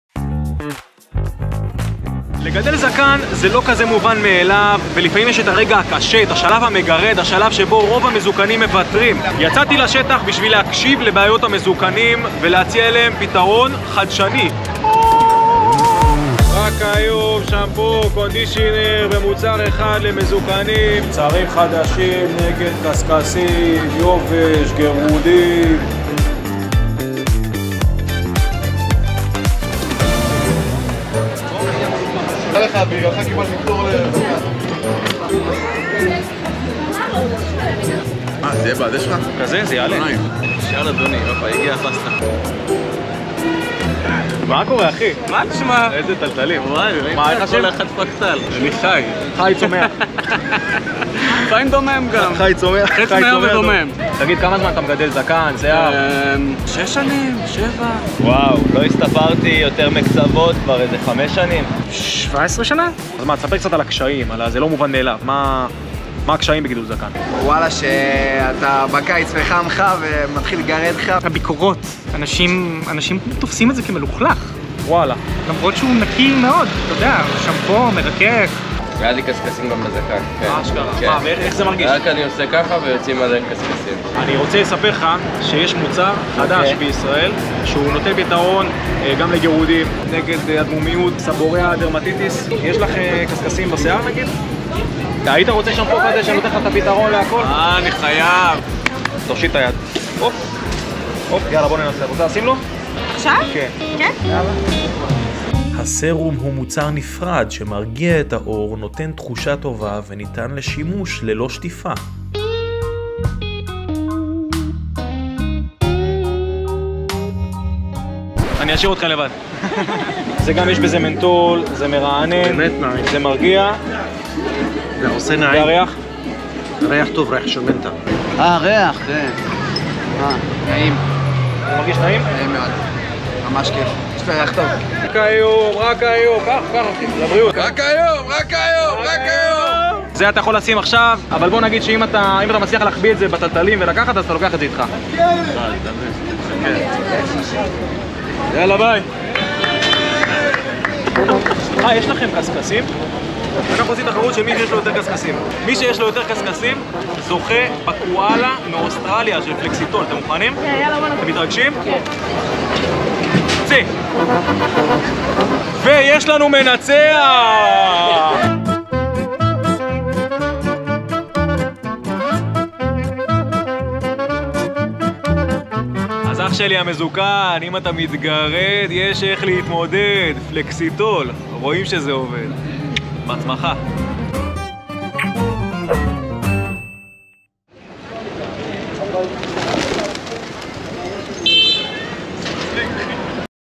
גלעד המזוקן יוצא להילחם בגירודים
אפשר להפסיק להתגרד!
יצאתי לשטח להקשיב לבעיות המזוקנים,
ולהציע להם את הפתרון החדשני של פלקסיטול.
רוצים להתייעץ איתי על כל מה שקשור בזקן?

גירודים בזקן
מה לעשות כשמגרד בזקן
מגרד לי בזקן
פלקסיטול של אלטמן
שמפו נגד גירודים שמפו נגד קשקשים